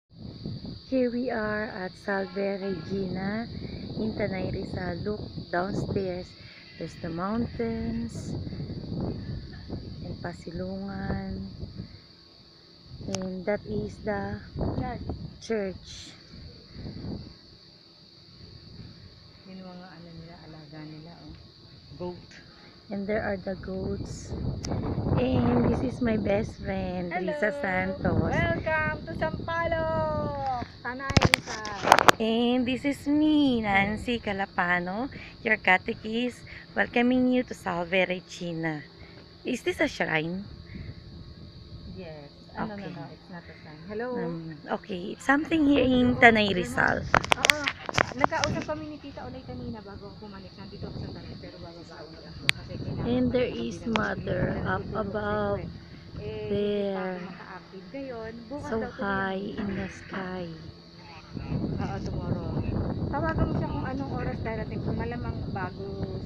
Visit to Regina Rica
With Riza Omana at RR, Sampaloc, Tanay, Rizal, Philippines